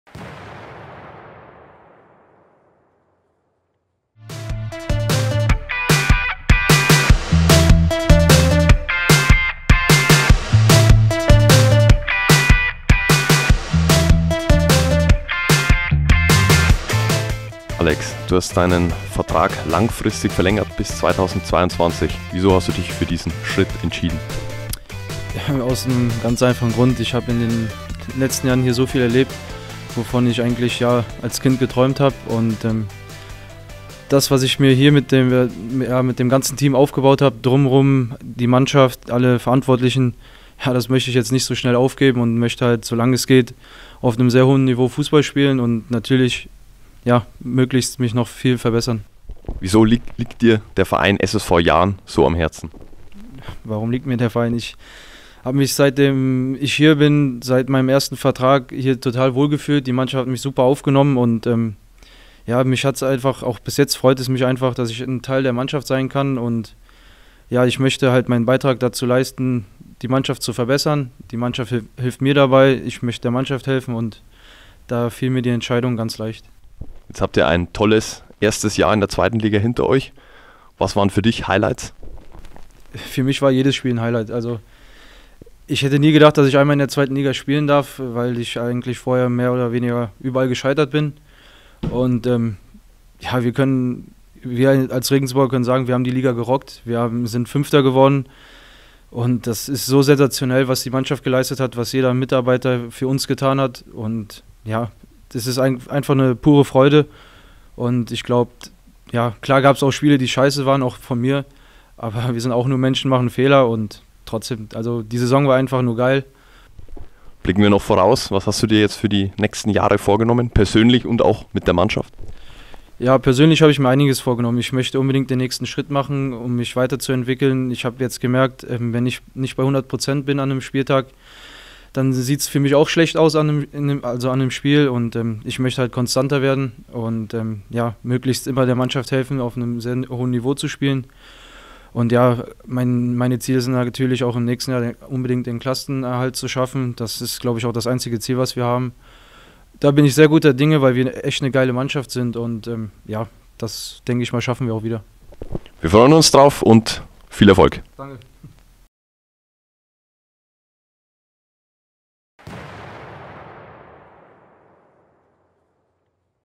Interview mit Alex Nandzik
Linksverteidiger Alexander Nandzik verlängert seinen bislang bis 2019 datierten Vertrag um weitere drei Spielzeiten. Er läuft damit bis 30. Juni 2022 für den SSV Jahn Regensburg auf.